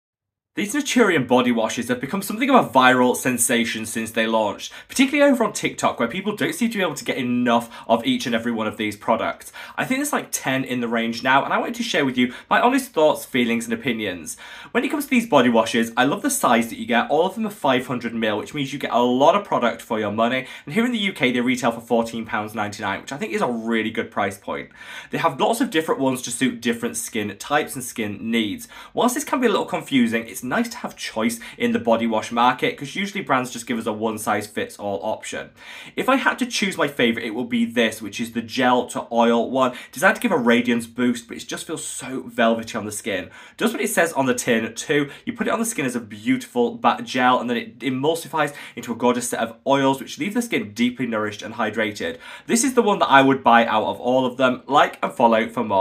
This Body Wash Has Gone Viral - TESTING NATURIUM BODY WASHES
Lets take a look at the viral Naturium body washes and whether they are worth the hype which they seem to be getting. Naturium review.

Hey guys and welcome back to Mad About Skin where in this video we are talking the Naturium body washes and whether this viral skincare brand is actually worthy of they hype they are getting.

Body washes usually come in a one size fits all form with little choice in the market and this is a shame as the skin on our body needs the same love as the skin on our face gets.

Lets take a closer look at the body washes by naturium and work out whether any of them are worthy of a place in our shower routine.